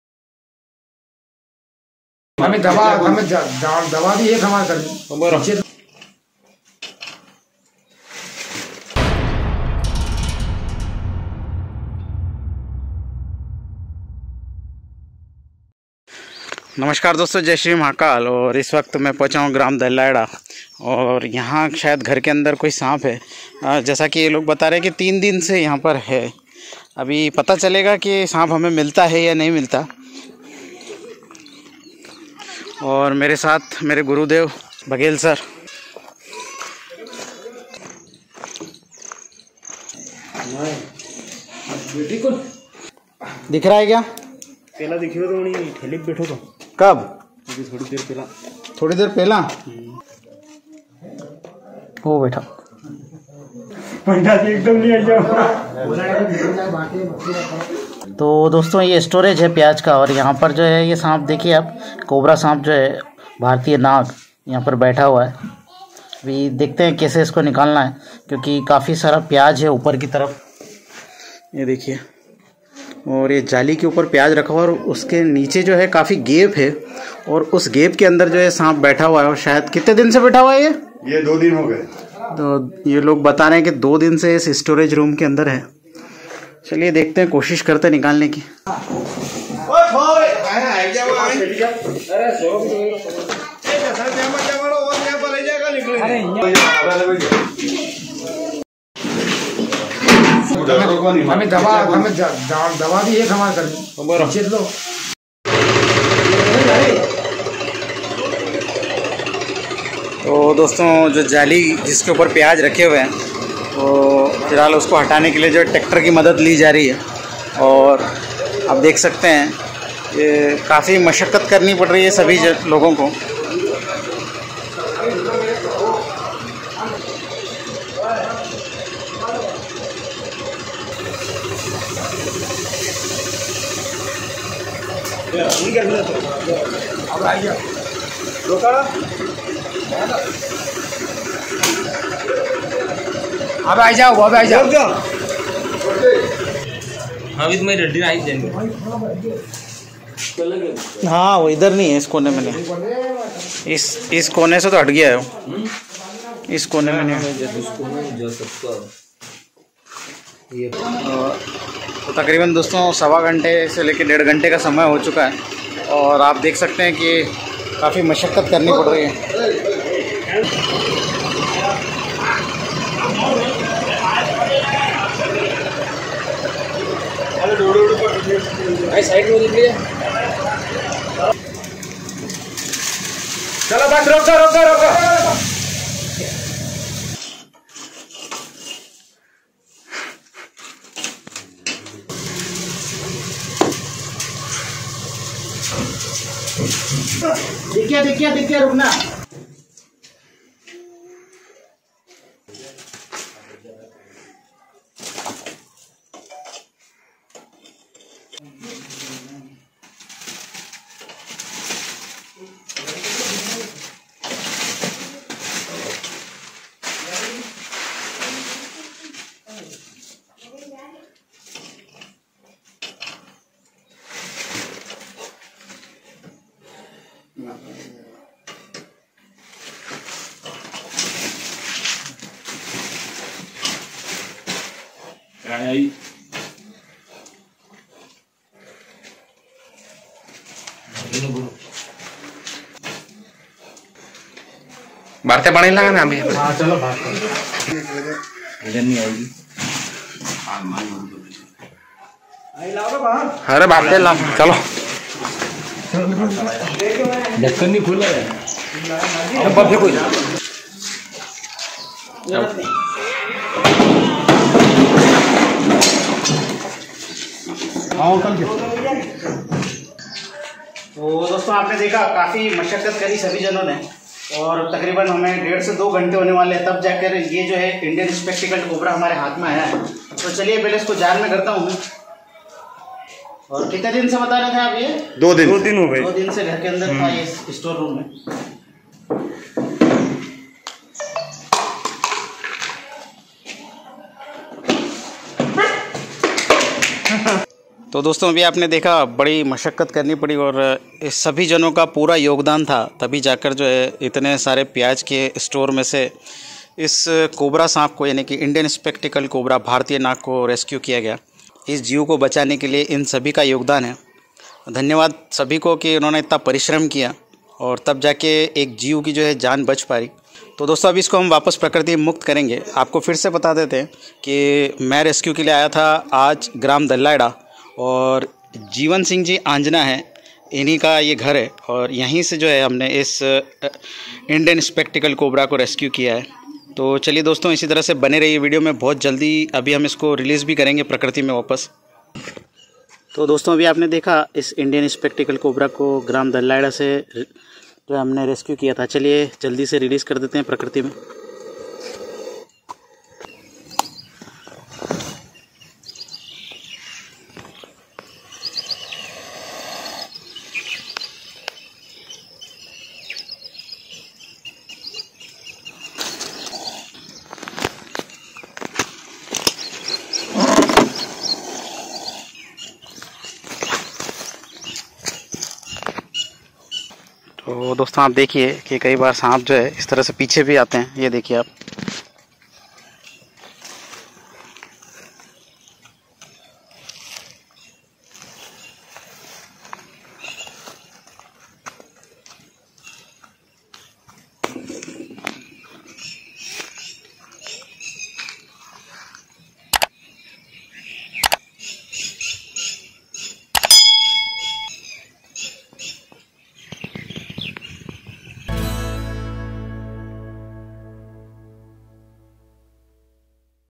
प्याज के गोदाम में घुसे कोबरा सांप को किस तरह निकाला गया Rescued Indian Cobra Snake | S05R063
AMIT SHARMA
Nagda, Madhya Pradesh ( India )